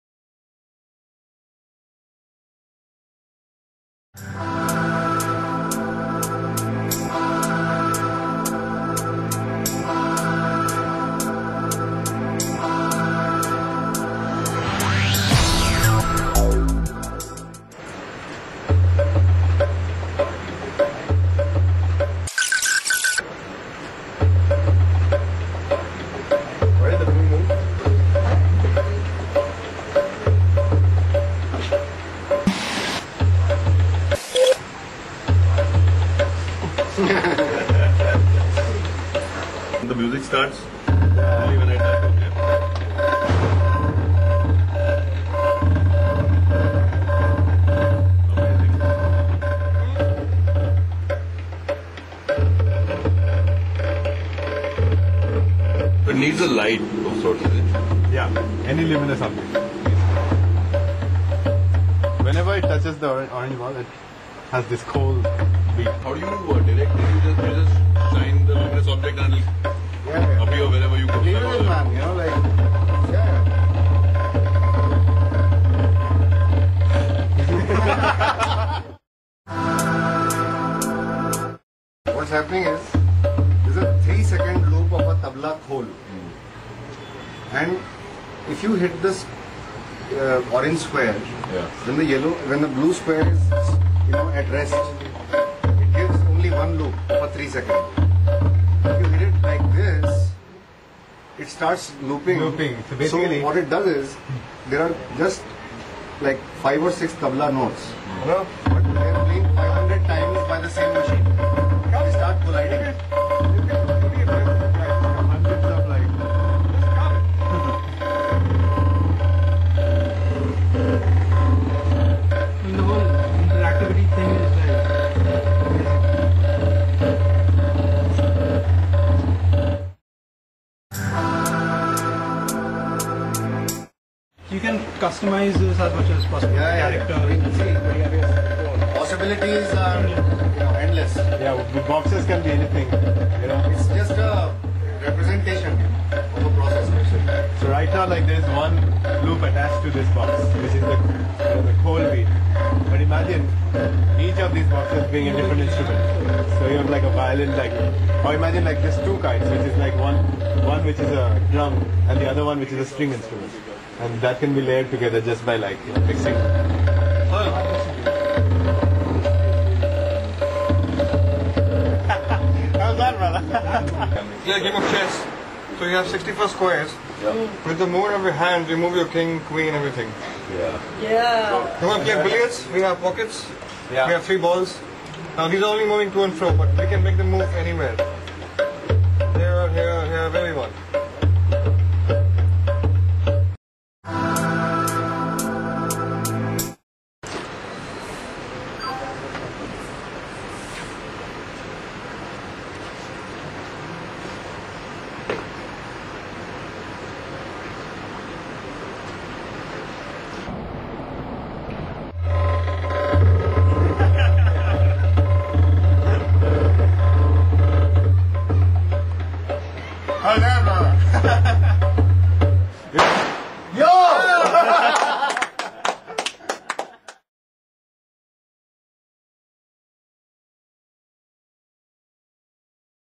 parab0xx by DJ Fadereu
parab0xx is a software prototype that shows how you can interact with virtual images projected on a simple living room table, using a webcam. You can play games, make music, or even edit videos. In short, it is a kind of primitive computer that obeys the instructions of a simple LED (light-emitting diode). Alternately, you can use a luminous phone screen, a candle's flame, or even a piece of white paper as a cursor.